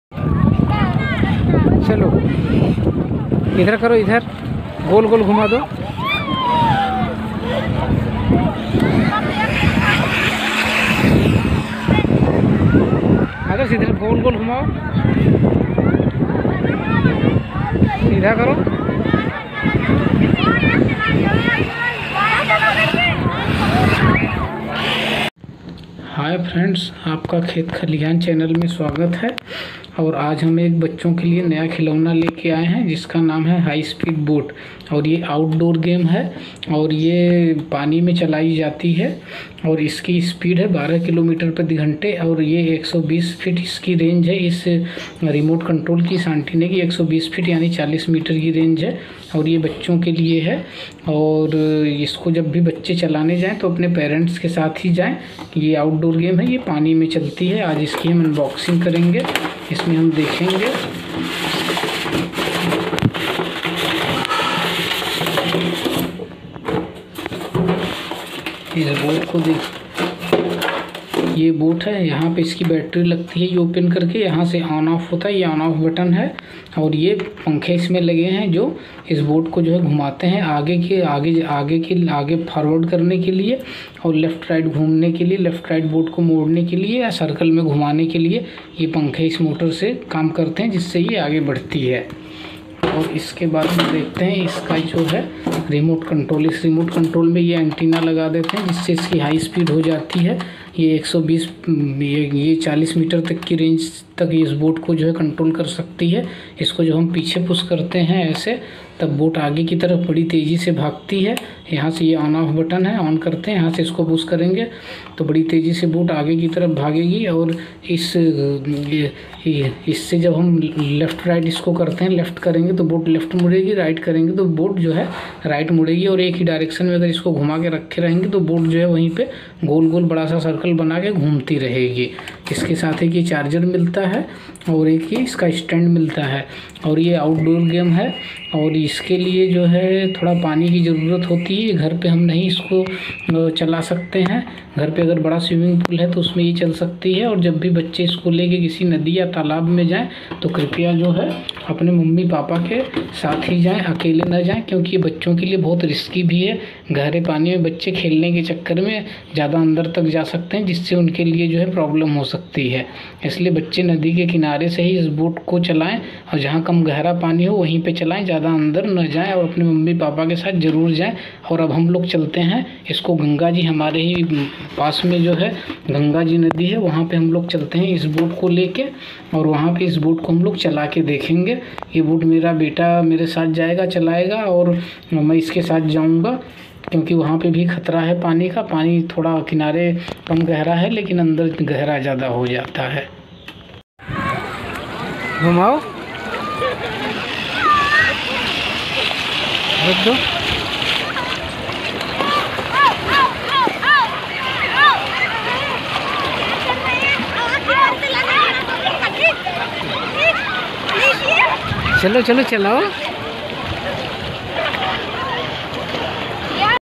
High Speed kids water boat toy for kids| New Toys (हाई स्पीड बोट टॉय) Unboxing and demo
ये एक हाई स्पीड बोट टॉय है | इसको बच्चो को अकेले पानी में चलाने के लिए न दे | बच्चो के साथ किसी न किसी बडे पेरेंट्स का होना बहोत जरुरी है |
kids toys
unique kids toys
water toys for kids
New Toys for kids

Price : 1500 INR
purchased location : chadni chowk new delhi.